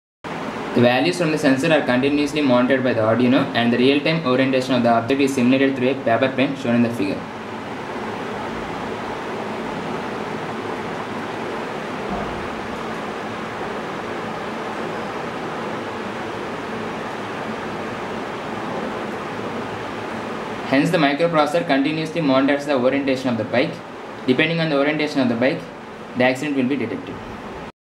Accident Detection Module 2
This is the explanation of how the accident detection module that we used in our project worked.

Here we show the 3 pitch, yaw, roll being sensed, and the sensed values are used to simulate a virtual plane.

We use the Arduino UNO board
) as the microcontroller board, and the sensor used is MPU - 6050

Credits -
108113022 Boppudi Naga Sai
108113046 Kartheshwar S